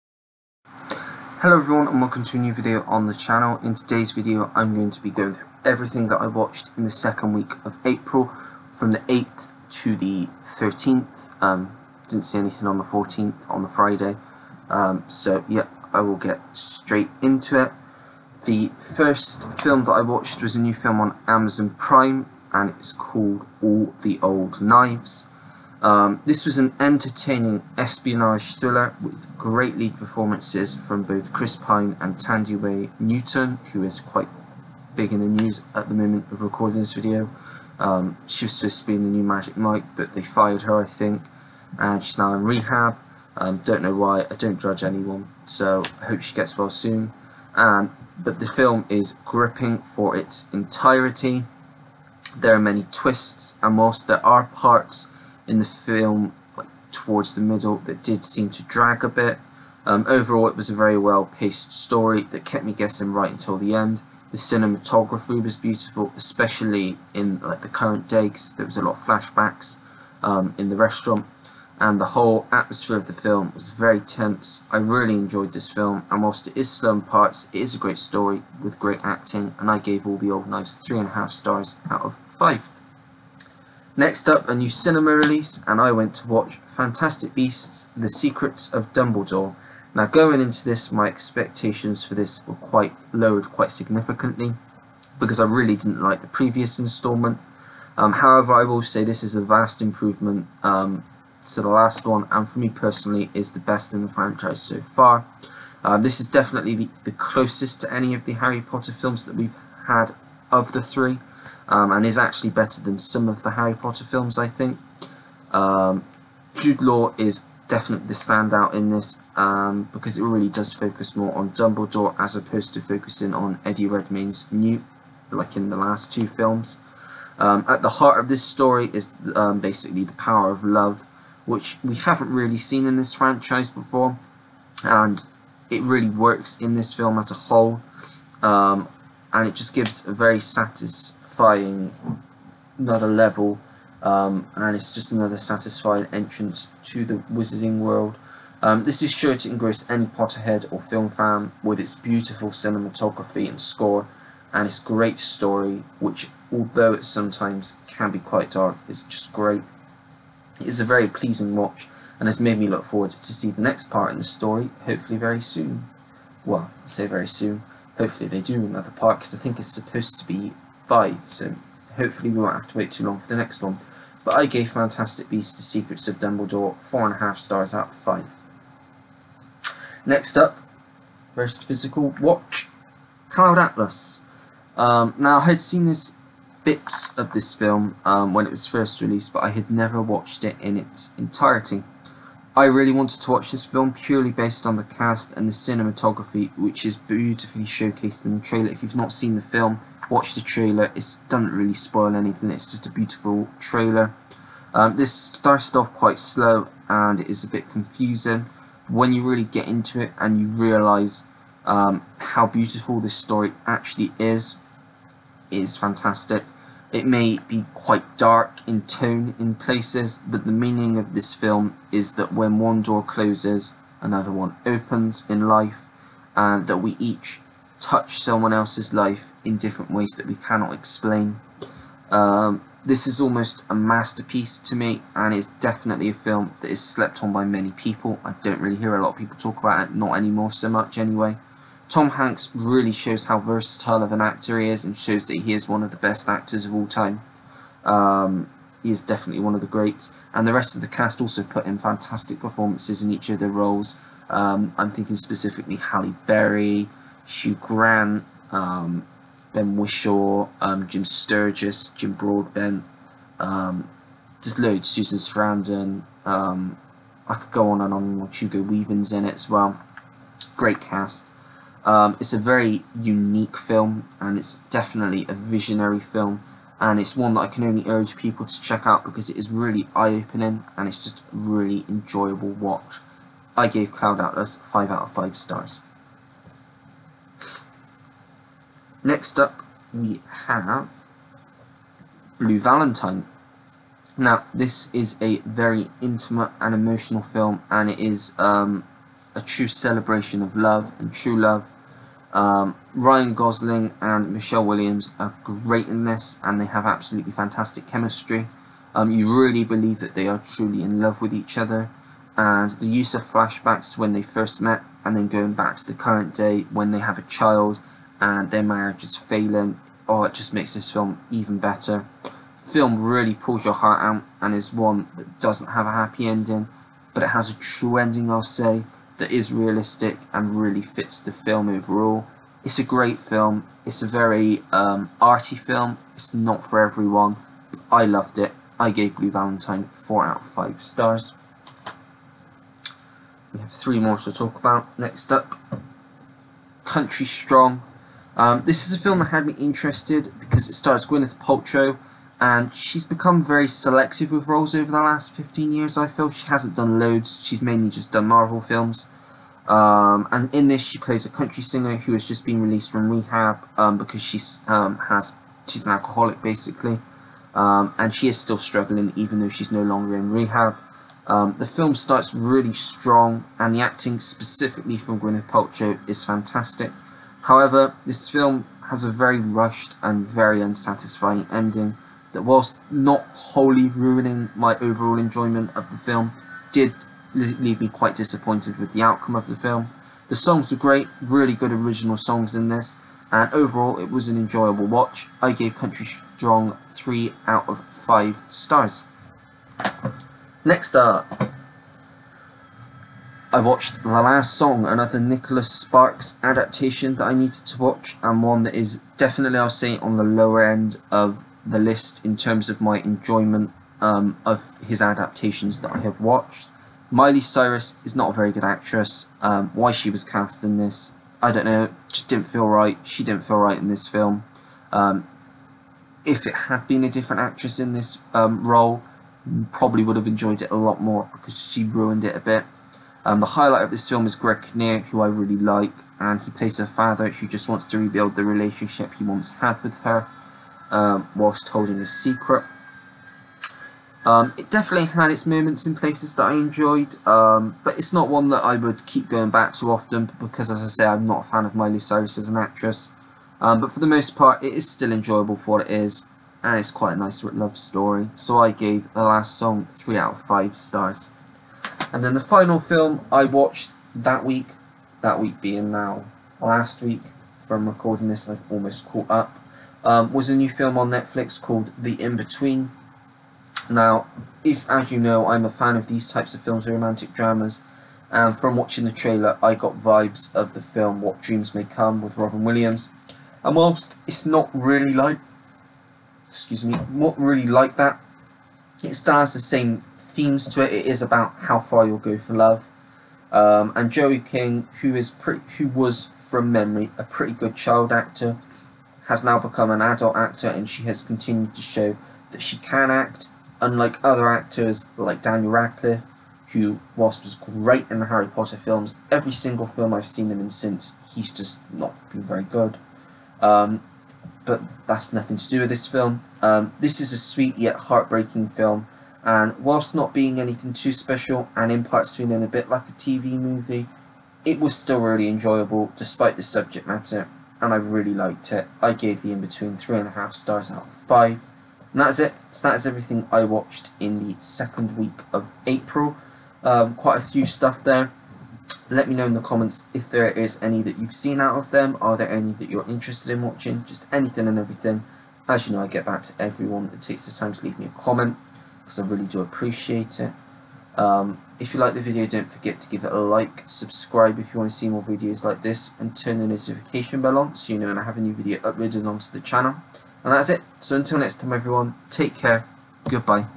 Week 2 (8th-14th) April 2022 Reviews
These are all of the films that I watched in the second week of April 2022.